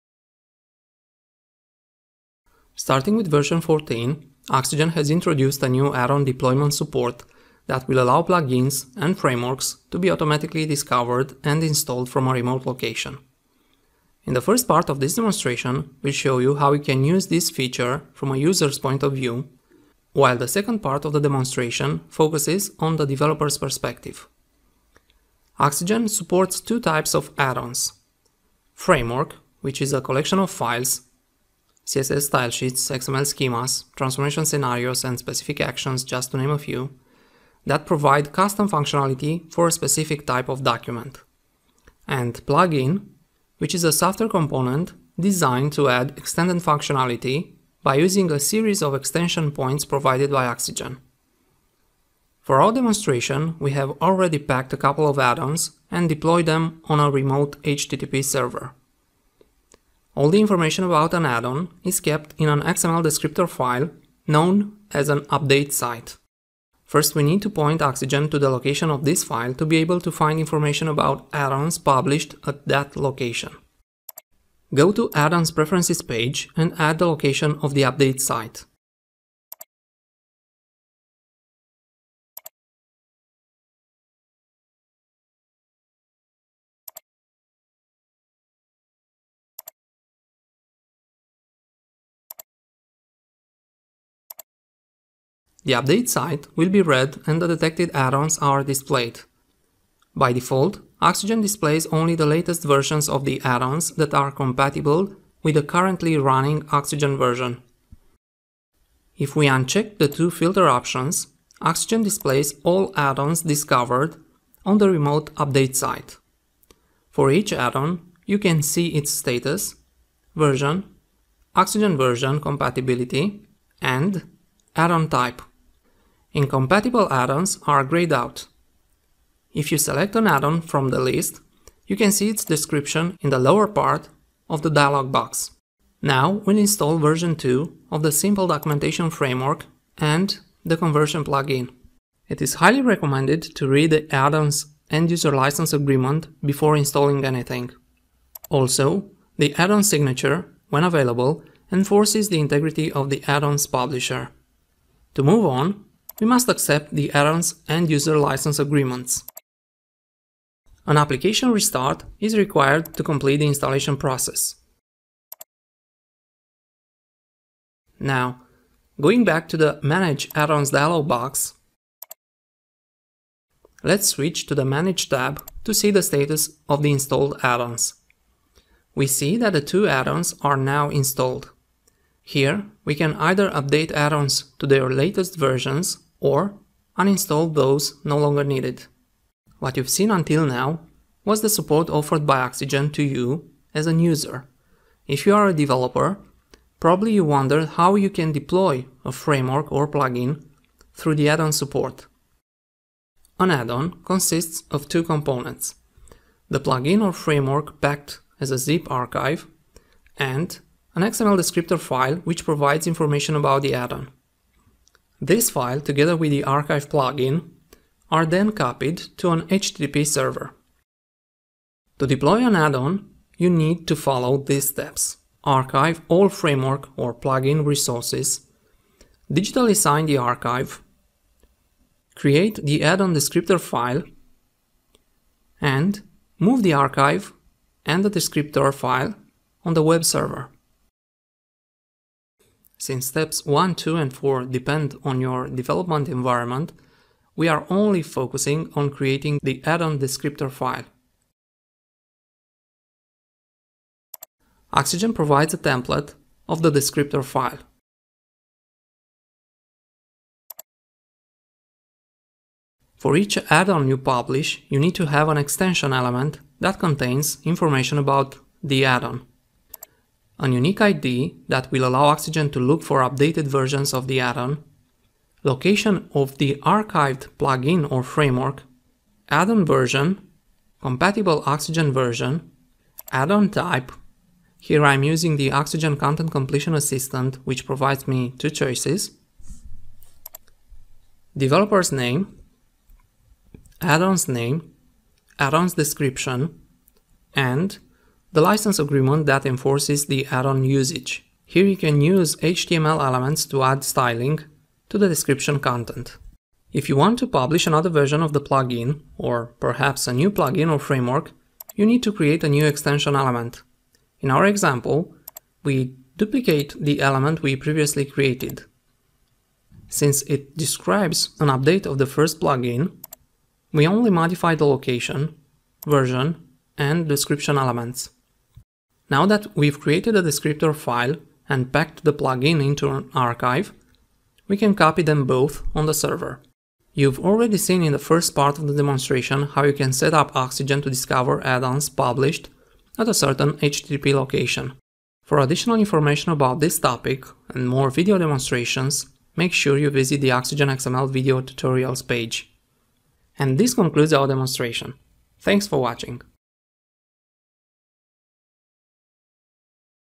oXygen XML Editor - Add-ons Support
This video demonstration presents the new add-on deployment support that will allow plugins and frameworks to be automatically discovered and installed from a remote location.